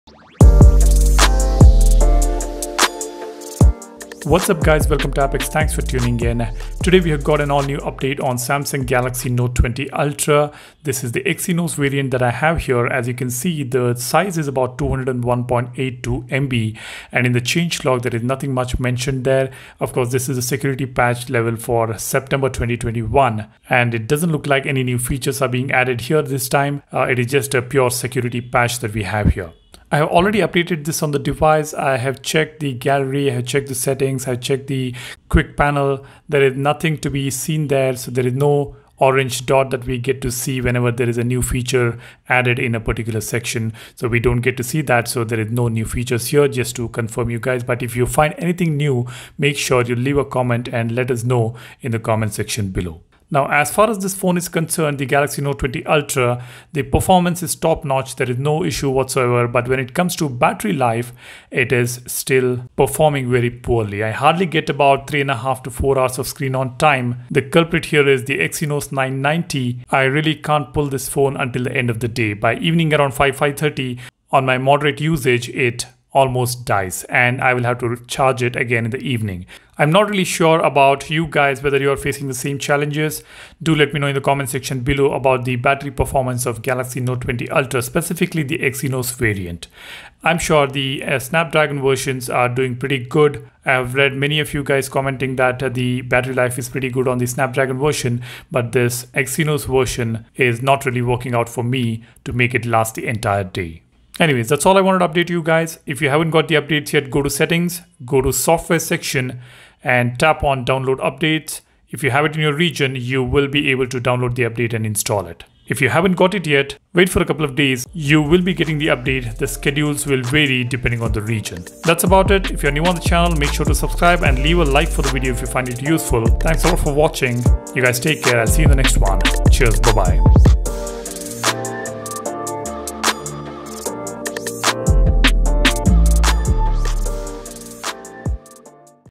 New update - Samsung Galaxy note 20 series - One UI 3.1.1
The latest September security patch is here for Samsung galaxy note 20 ultra exynos variant running on One UI 3.1.1